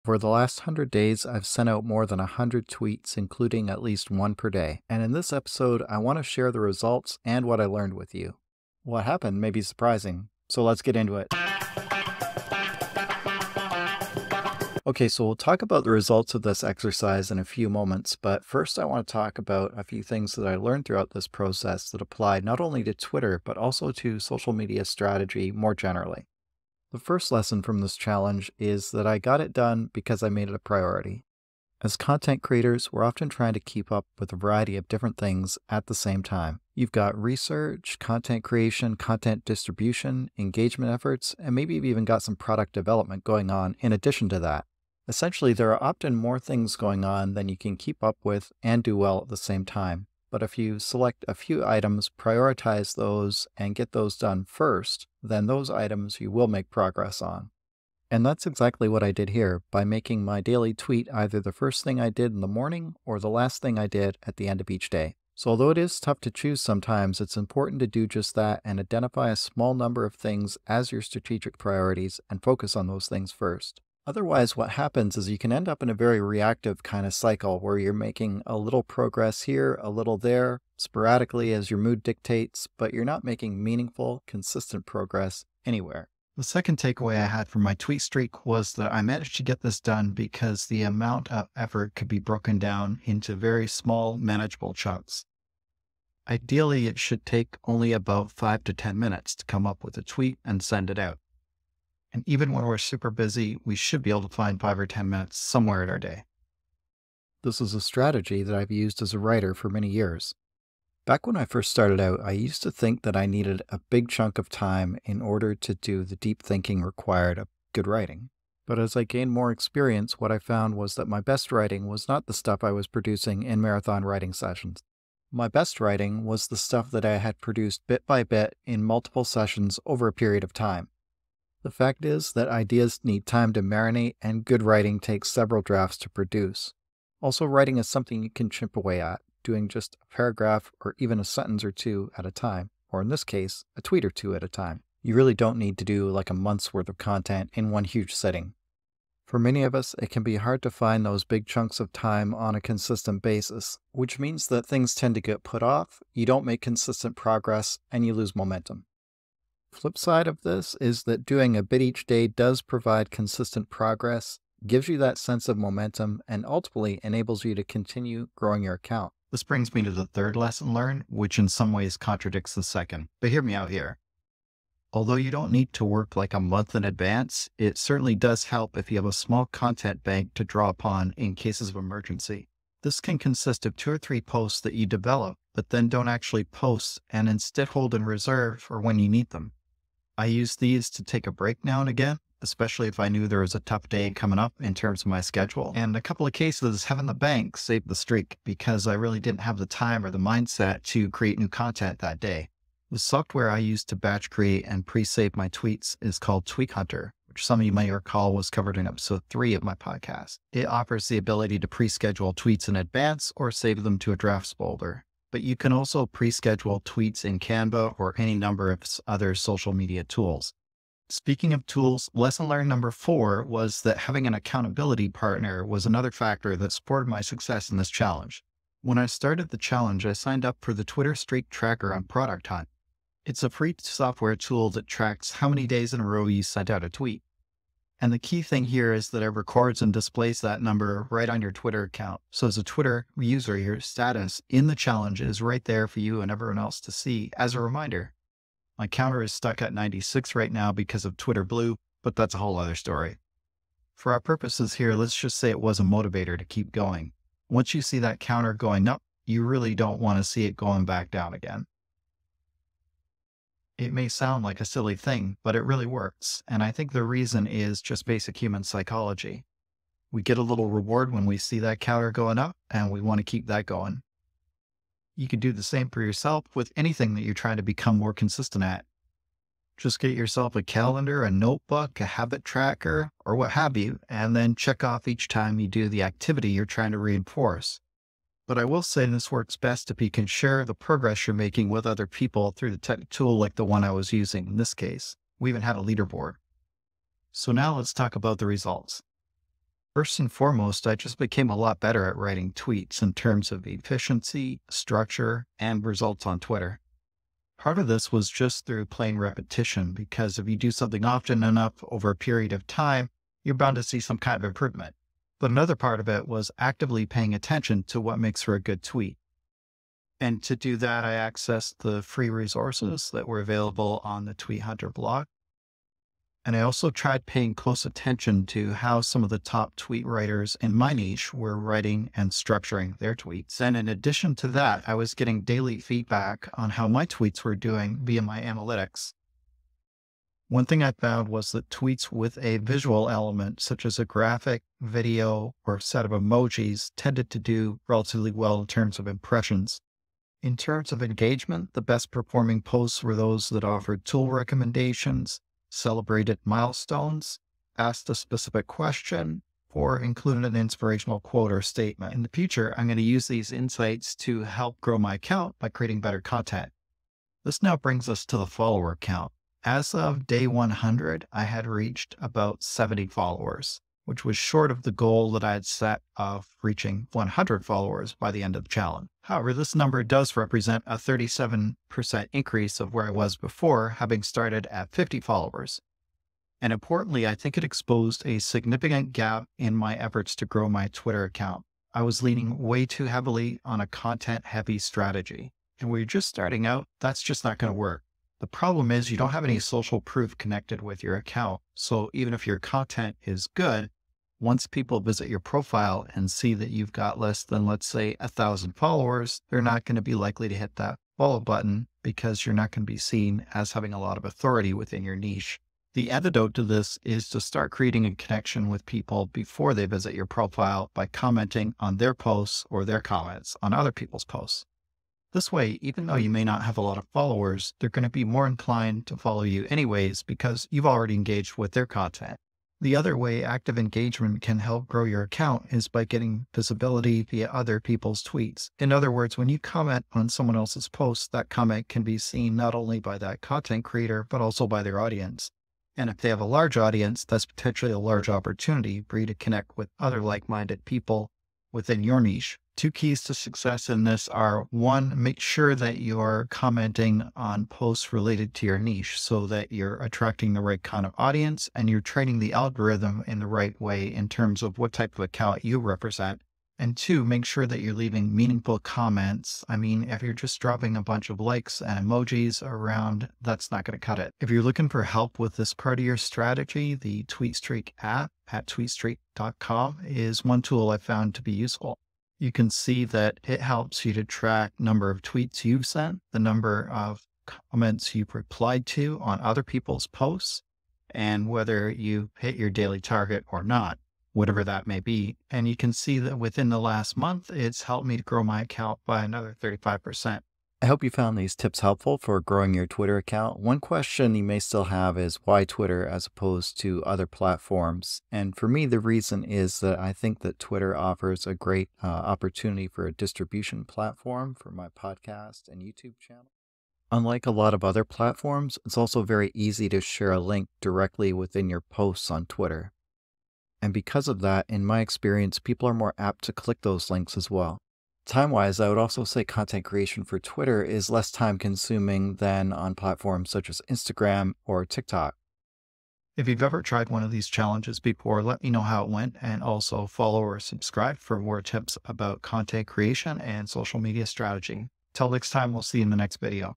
My 100 Day Tweet Streak! (Results May Be Surprising).
Have you ever wondered how much you could grow your Twitter account by taking on the 100-day tweet streak challenge? That's where you aim to send out one tweet every day for 100 days straight.

Well, I tried this out myself. And in this episode, I'm sharing all the results and everything that I learned along the way.

Video Chapters
0:00 Introduction
0:26 Lesson 1: What you prioritize gets done
1:26 Lesson 2: Manageable work packages are your friend
2:49 Lesson 3: A content bank can save the day
3:41 Lesson 4: Get yourself an accountability partner
4:57 Result 1: Better Tweets
6:07 Result 2: Follower growth via content
6:55 Result 3: Follower growth via engagement
8:20 Conclusion

Twitter can be a great platform for distributing your podcast or YouTube channel. Check it out and let me know how that goes in the comments, or share your experience if you've ever tried a 100-day social media channel such as this one.